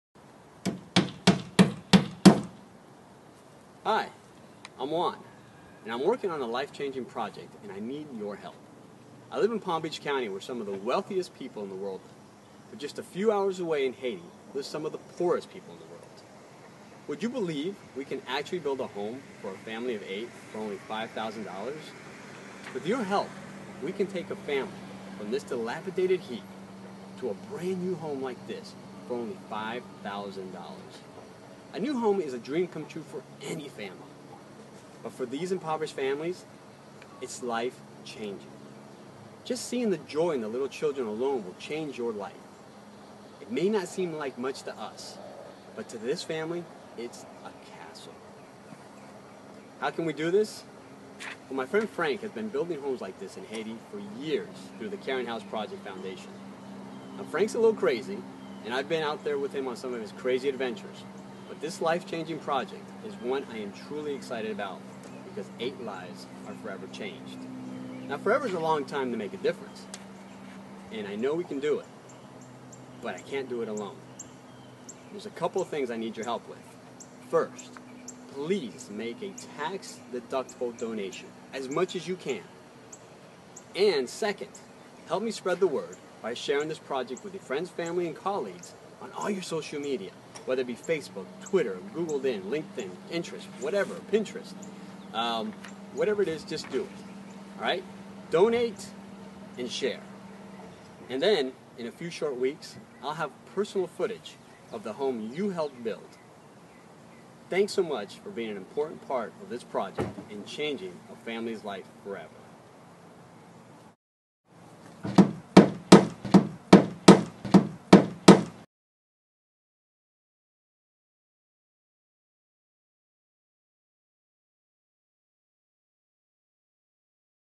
BTEPB to Build a Home for a Family of 8 in Haiti
Hey YouTube amigos, I have the opportunity to build a home for a family of 8 in Haiti, in one of the poorest areas in the world.  As you can see in the video, many families live in shacks (for lack of a better word to describe unsafe structures).

We are building a concrete home with a real foundation.  To this family, it's a dream come true...a castle.  I'm honored to be a part of this project but can't manage it alone.

My friend Frank of the Caring House Project Foundation heads up building the village where these homes are constructed. He's done it for years and the village provides these new homeowners with running water, farming area, community center and more, creating a real self-sustaining community. truly helping the poor provide for themselves.

Your role in this is financial support...with your tax deductible donation, we can build the next home in this village and take a family of 8 out of the "shack" and into the "castle," including their little children.

No one in the world should live like this and in Haiti, $1 goes a long way.  $5,000 builds a home that changes a family's life.  We can do this one family at a time and this time, I need your help.

Please donate to this worthy cause and help make a huge difference in this family's life.

p.s.  I'm also going to discuss using the BTE method at the villages.